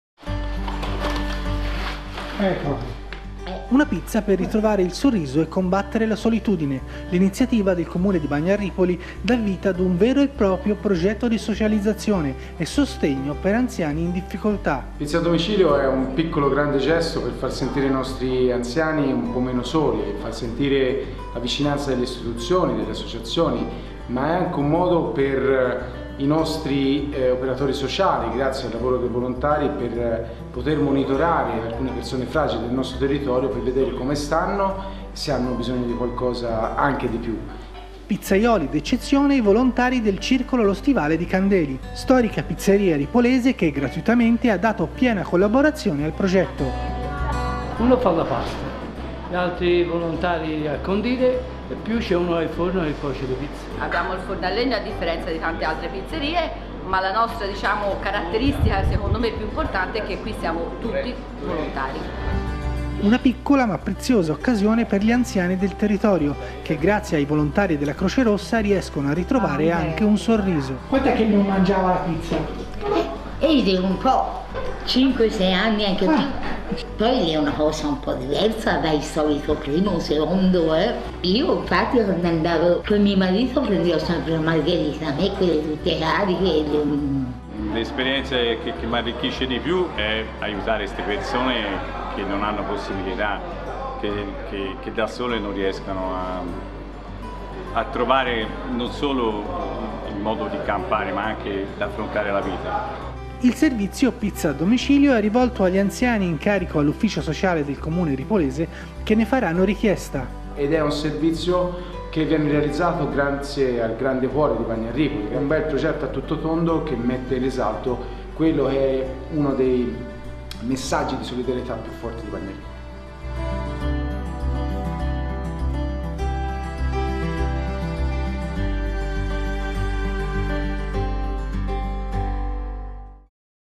Una Pizza per ritrovare il sorriso
L’iniziativa del Comune di Bagno a Ripoli, da vita ad un vero e proprio progetto di socializzazione e sostegno per anziani in difficoltà.
“Quello con la pizza a domicilio, come riferiscono i volontari, per gli anziani è già diventato un appuntamento fisso e molto atteso” – dicono il sindaco Francesco Casini e la vicesindaca Ilaria Belli -. “Tanto che se qualche volta agli operatori è capitato di ritardare alcuni minuti, gli anziani hanno contattato la centrale della Cri per sapere se era accaduto qualcosa. Chi porta la pizza spesso viene fatto accomodare per scambiare due parole sulla vita di tutti i giorni.”
Pizzazioli d’eccezione i volontari del Circolo lo Stivale di Candeli, storica pizzeria ripolese che gratuitamente ha dato piena collaborazione al progetto.
Un piccola ma preziosa occasione per gli anziani del territorio, che grazie ai volontarei della Croce Rossa, di Bagno a Ripoli, riescono a ritrovare un sorriso.
Il servizio Pizza a Domicilio è rivolto agli anziani in carico all’ufficio sociale del comune ripolese che ne faranno richiesta.